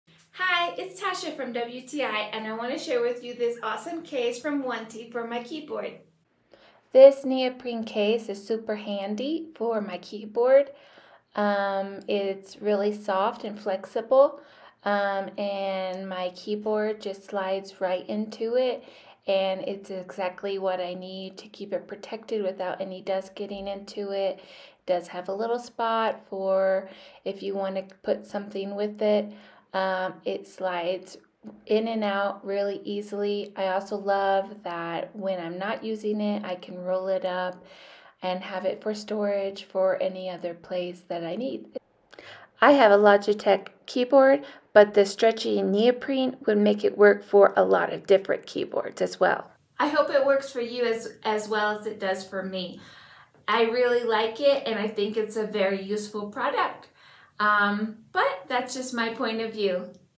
Wanty Keyboard Bag Sleeve Protectors | Our Point Of View
Wanty Keyboard Bag Sleeve Protectors

(Commissionable Links)

Speciall design for Logitech MK270 Wireless Keyboard and Mouse Combo for Windows, QWERTY
Made of neoprene to protect your keyboard from dust, water and scratches
Large compartment for Logitech wireless keyboard & outer mesh pocket for your other small accessories
Just a protection bag, keyboard and accessories not include.
Dimension: (L X W X H) 17.59 X 5.9 X 0.39 inch